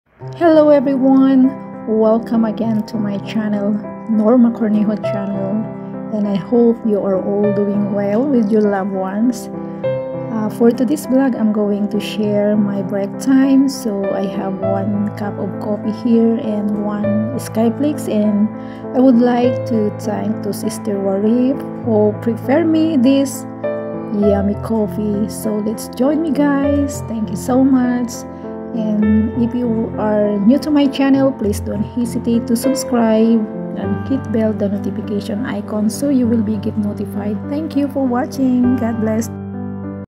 Thank you to my colleague|Late upload |Norma Cornejo
Just wanna share the yummy coffee that prepared by my colleague for me for a break time.Thank you sis Wareef,you are so thoughtful. May God bless you.shortvideo#